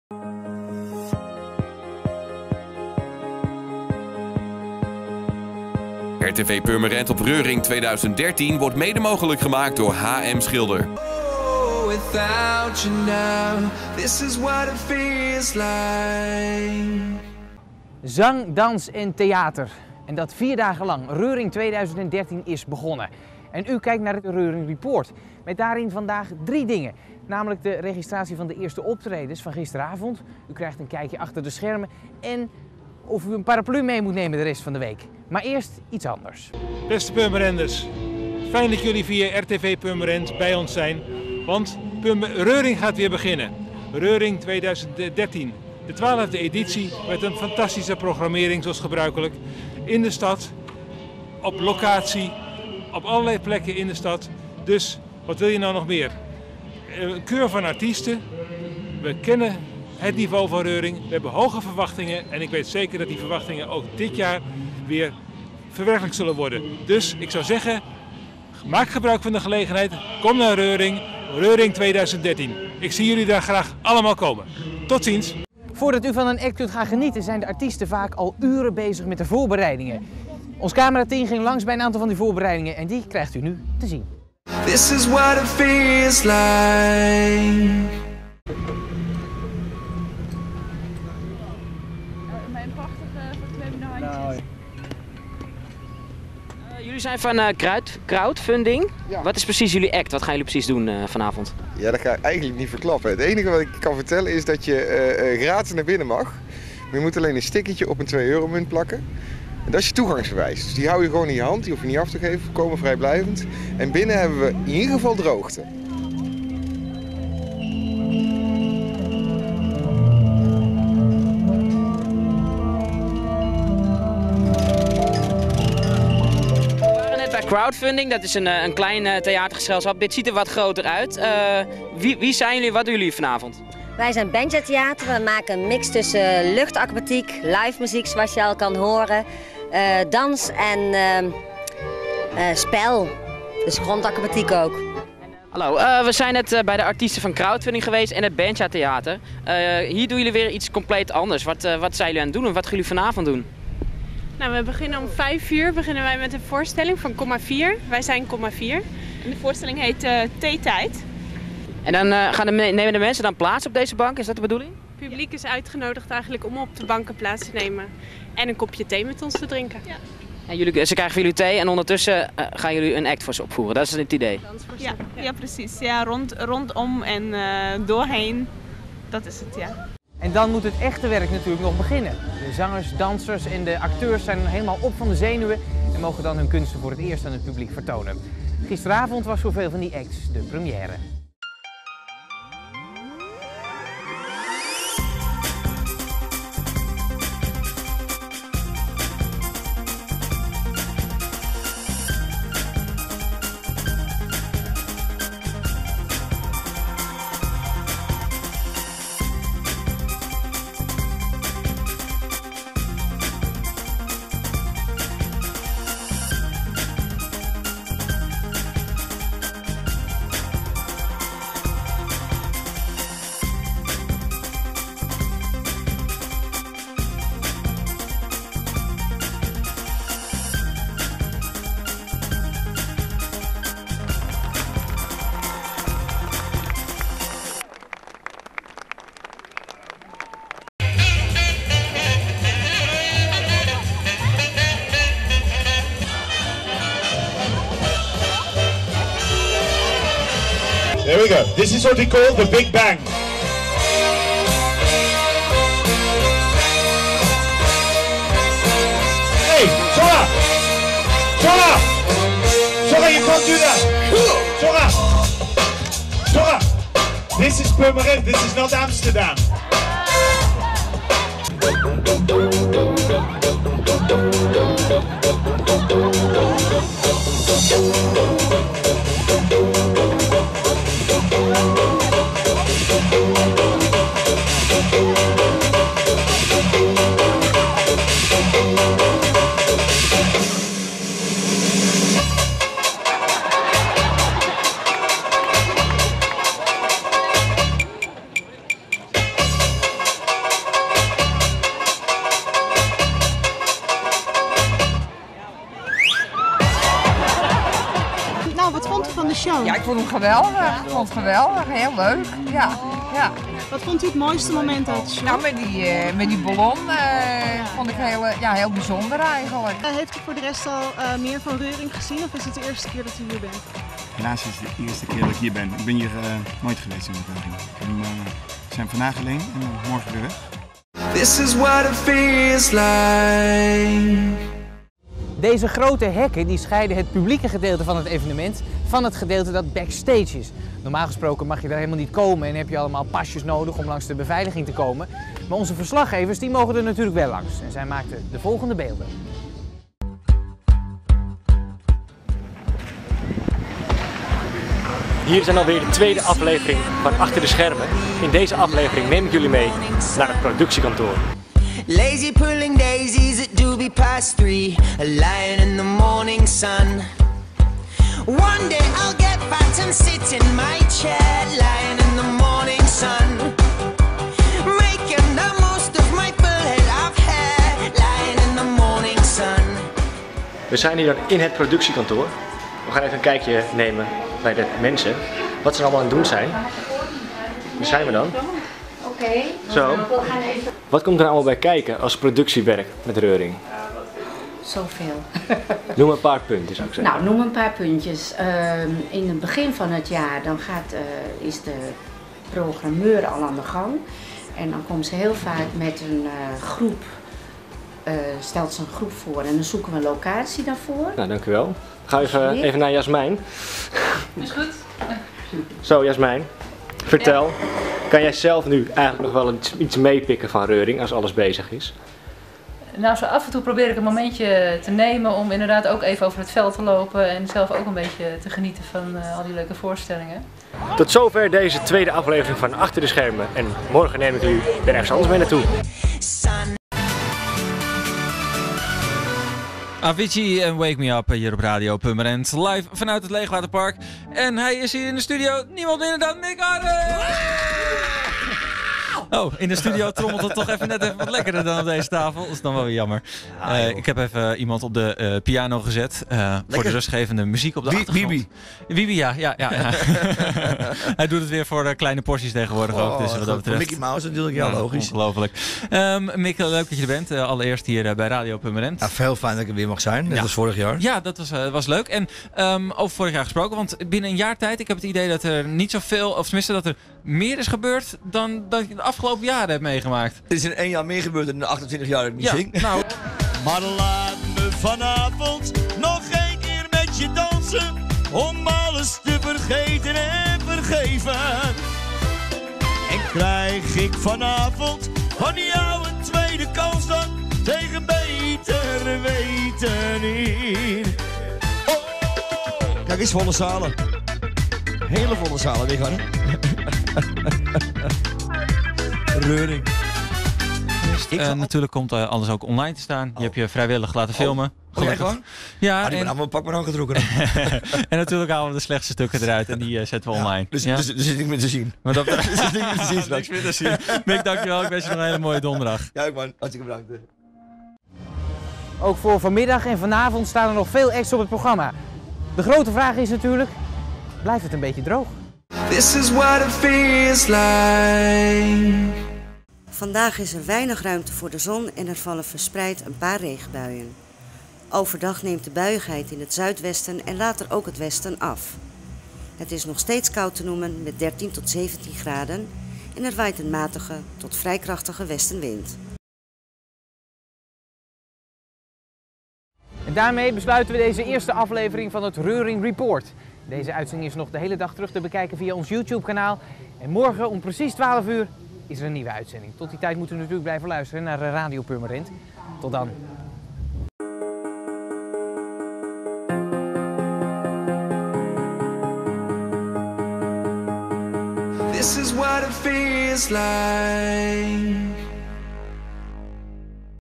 Reuring Report 2013 - Donderdag 27 juni 2013
RTV Purmerend zendt dagelijks live vanuit het Leeghwaterpark tijdens Reuring 2013 van 27 t/m 30 Juni.

Vandaag, Donderdag 27 Juni, brengen wij u het volgende: Backstage beelden, een boodschap van Burgemeester Don Bijl, diverse acts, Mick Harren bij Radio Purmerend en nog veel meer!